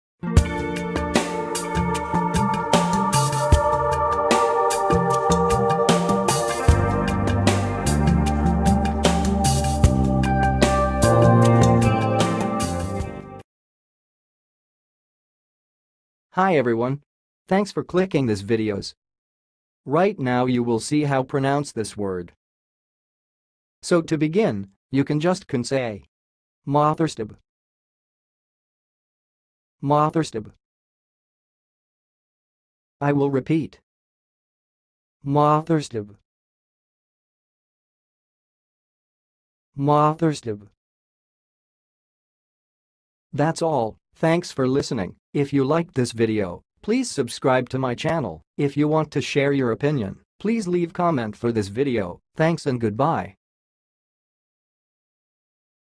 MOTHERSTOBE - HOW TO PRONOUNCE IT!?
Watch how to say and pronounce "motherstobe"!